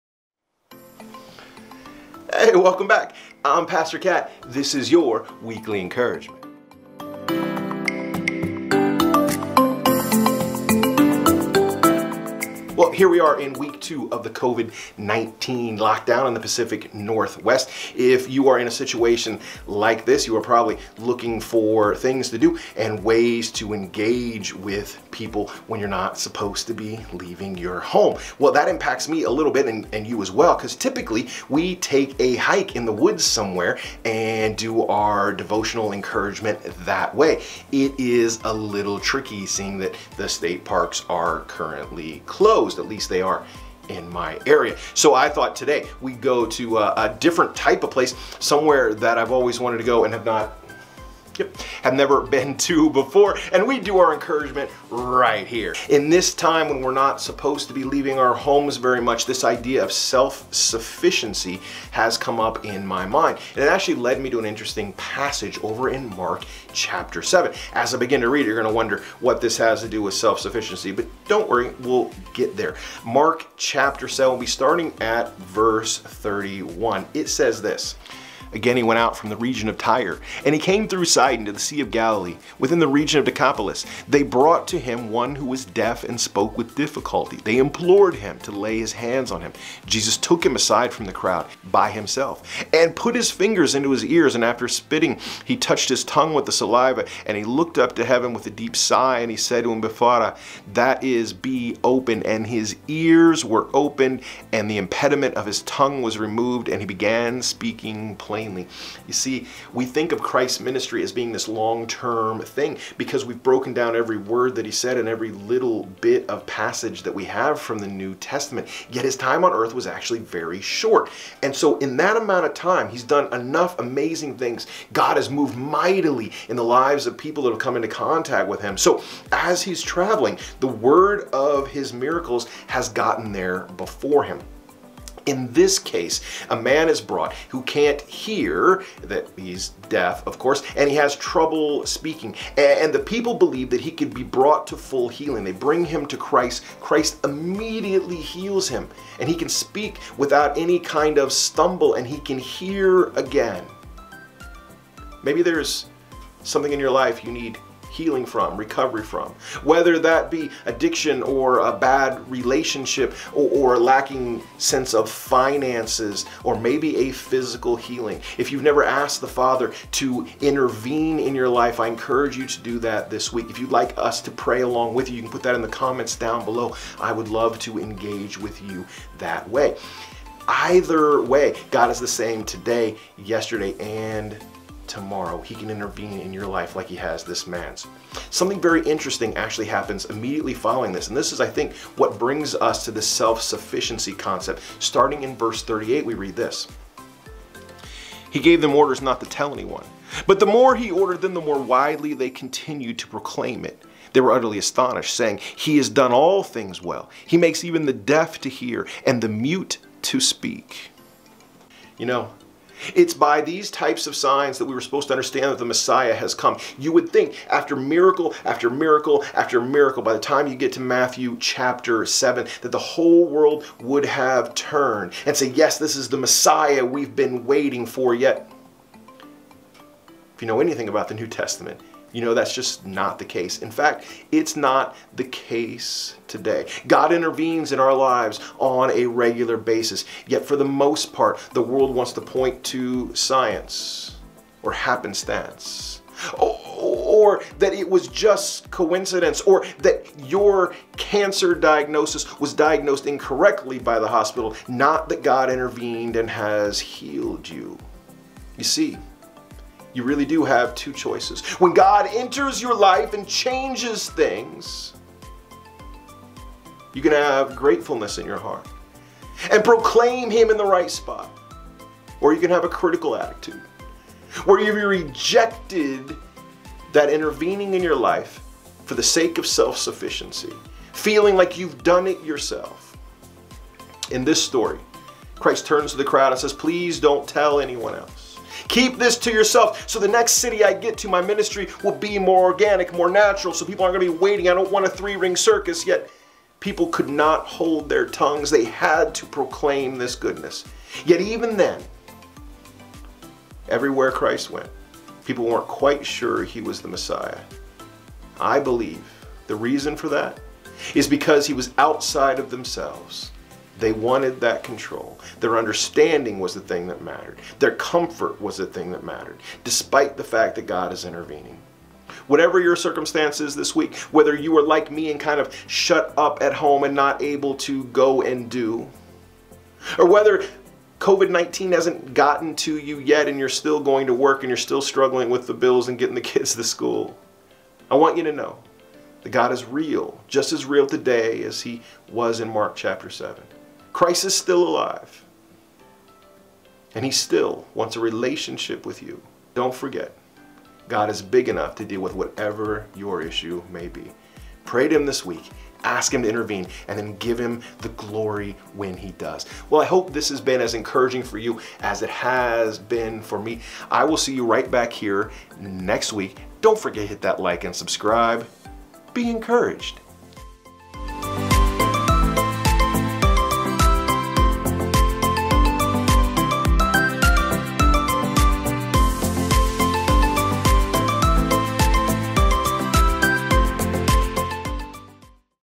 Self Sufficiency- Mark 7:31-37
This is a 5 Minute Devotional based on Mark 7:31-37

 Mark 7:31-37
31 Again He went out from the region of Tyre, and came through Sidon to the Sea of Galilee, within the region of Decapolis. 32 They *brought to Him one who was deaf and spoke with difficulty, and they *implored Him to lay His hand on him. 33 Jesus took him aside from the crowd, by himself, and put His fingers into his ears, and after spitting, He touched his tongue with the saliva; 34 and looking up to heaven with a deep sigh, He *said to him, “Ephphatha!” that is, “Be opened!” 35 And his ears were opened, and the impediment of his tongue was removed, and he began speaking plainly. 36 And He gave them orders not to tell anyone; but the more He ordered them, the more widely they continued to proclaim it. 37 They were utterly astonished, saying, “He has done all things well; He makes even the deaf to hear and the mute to speak.